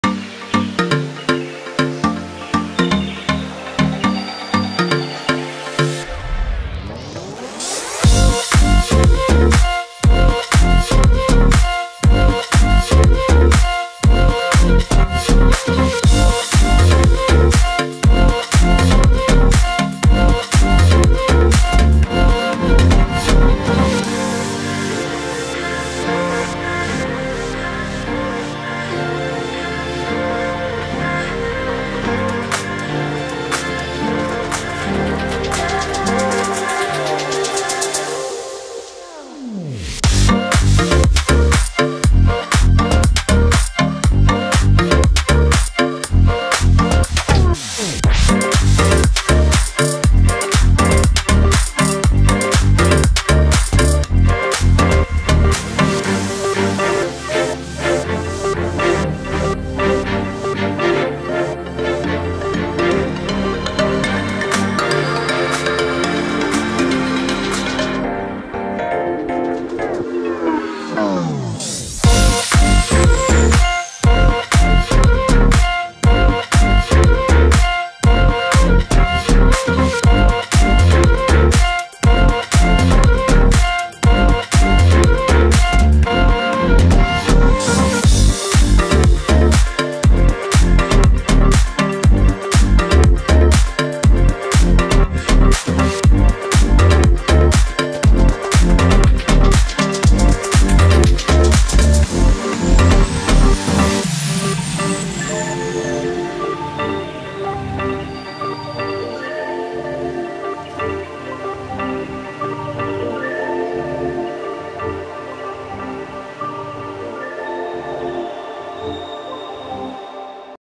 Scale Model Unboxing Renault Duster Diecast Car
"Hi Friends" 😊  Today unboxing 🎁 that we are going to see "Scale Model Renault Duster Diecast Car" I hope you like this video.